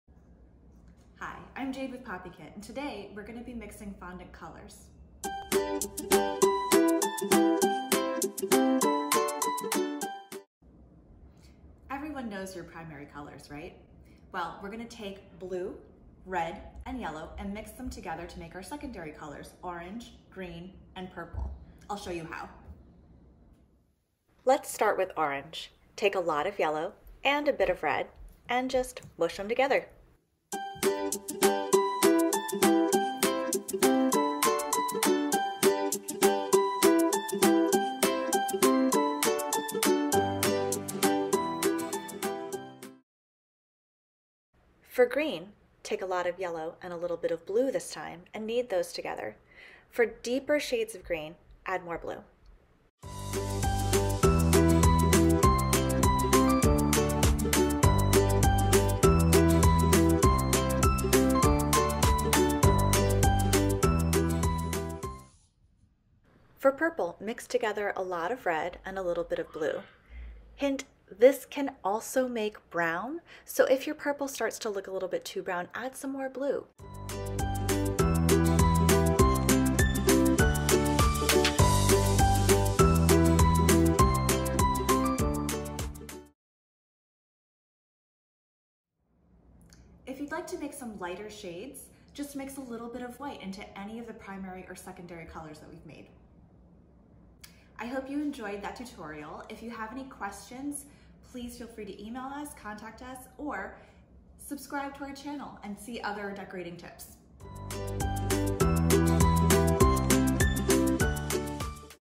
Fondant Tips: How to Mix Fondant Colors
How to mix together primary colors to create more colors - no need to spend lots of $ to get all the colors of the rainbow!